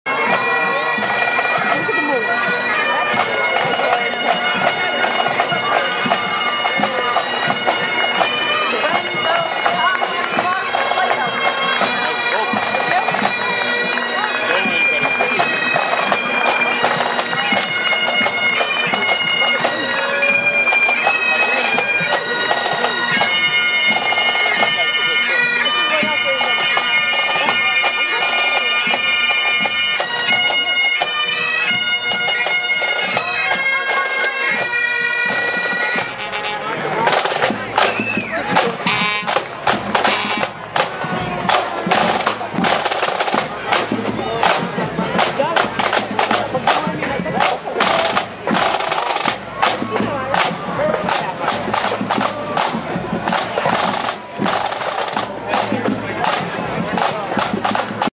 bag pipe warwick parade hx1 digital hd 1024 times 1080 full digital with sound warwick ri
full hd video from the hx1  ( may 30 2010) enjoy
the camera is on a tripod.  needs a tripod. have a great day.
great camera sony hx1 nice pictuers and video. I set video on fin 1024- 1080 AND WOW it is great .